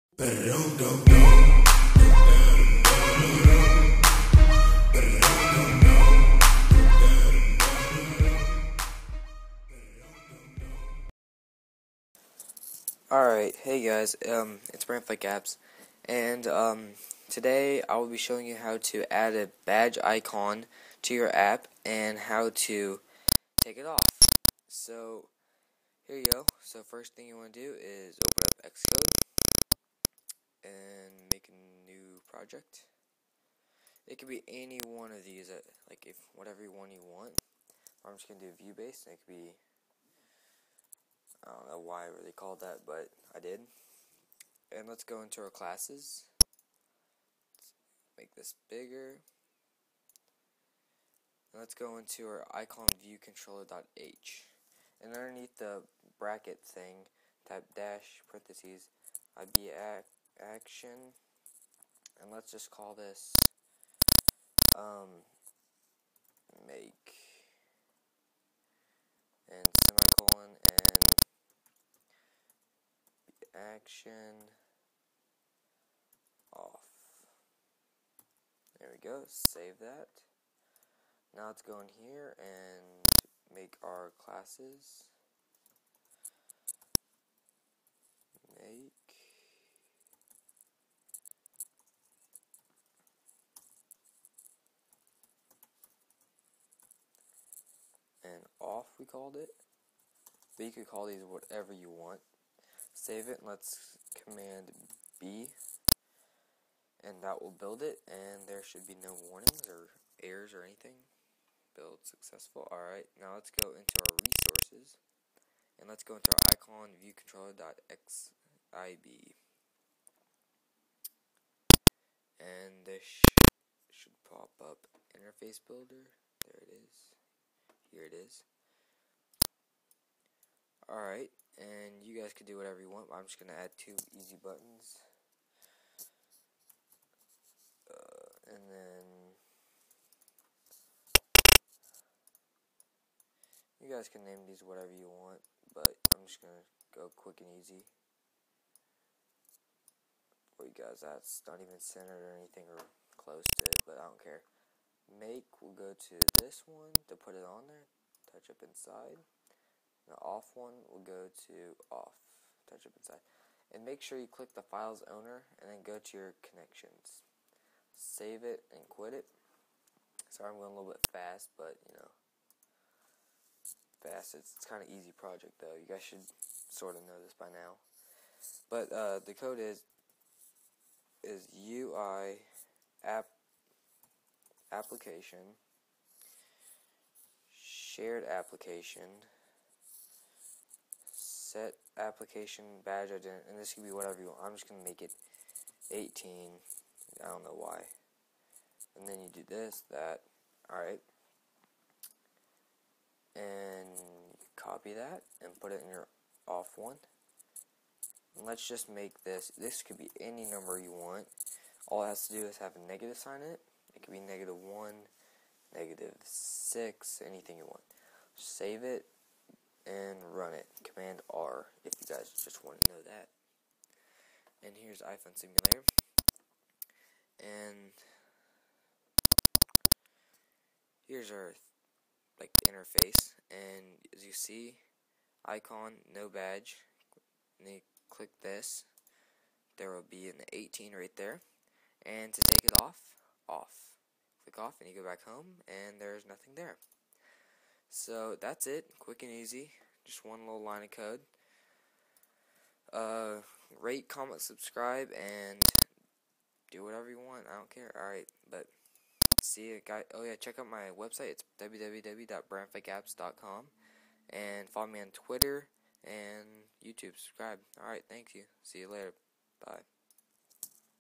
How to make a Badge on the app icon
This is how you make your app icon have a number with a badge. If you guy are to lazy and just want to copy and past it, hear it is:
[[UIApplication sharedApplication]setApplicationIconBadgeNumber:18];